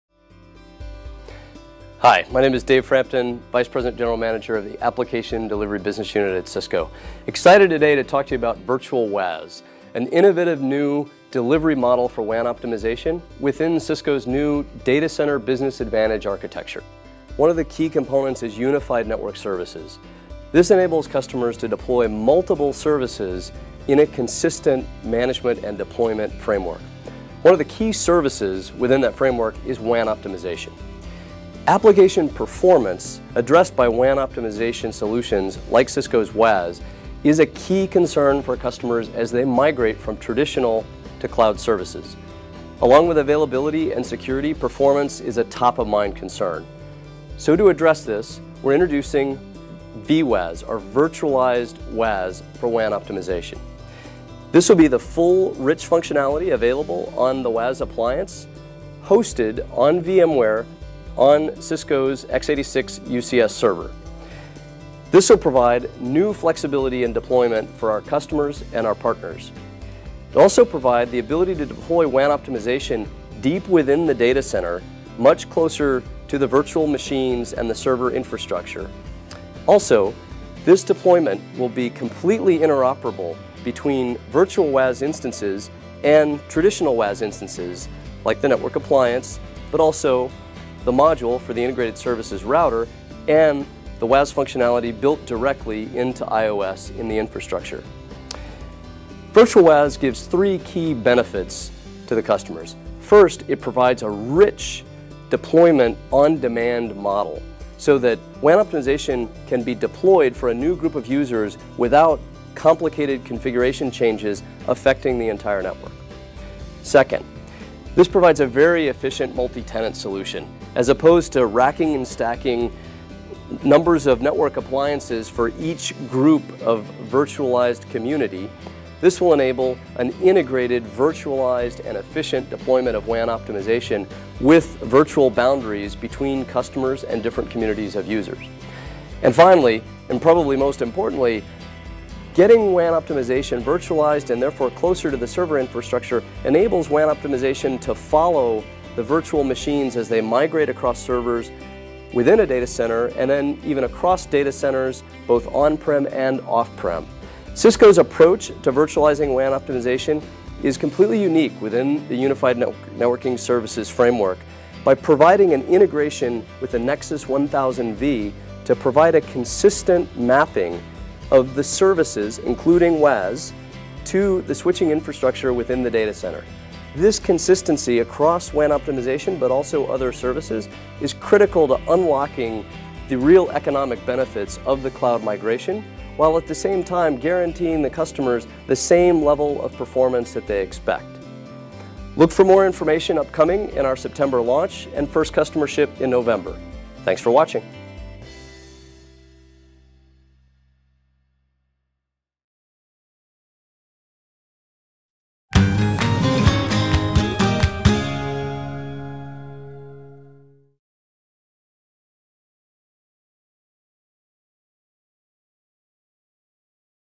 Cisco Unified Network Services and virtual WAAS
Cisco Dave Frampton, Vice President & General Manager , Application Delivery Business Unit talks about virtual WAAS, a new delivery model for WAN optimization in the Cisco data Center Business Advantage